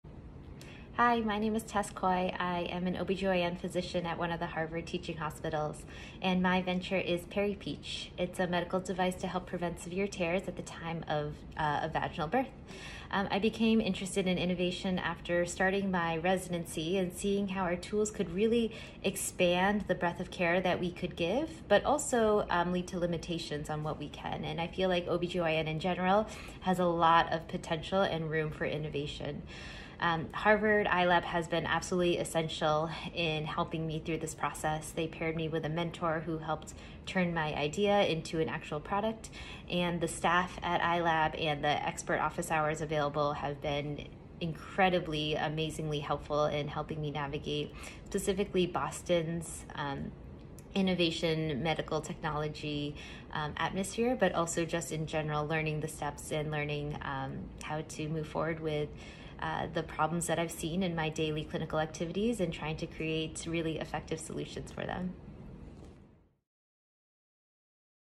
Meet PeriPeach, 2023 Harvard President’s Innovation Challenge Finalist
Tess Koi (Harvard Medical School) is the founder of PeriPeach, a venture focused on improving the comfort and care of birthing people. PeriPeach is a finalist in the 2023 Harvard President’s Innovation Challenge (PIC), an invitation to Harvard students and select alumni to propel their ideas forward — with the chance to win a share of $515,000 in non-dilutive funding, gifted from the Bertarelli Foundation.

Join us in person or virtually for the PIC Awards Ceremony on May 3 to hear the finalists pitch and celebrate the winners